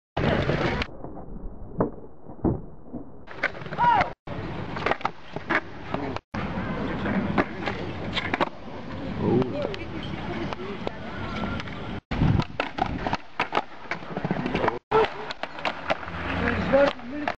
late backfoot flip!!!!!!
raphael vanhomwegen doet een late backfoot flip en freestylet een beetje en op het einde een paar nollie flips en een nollie flip nose manual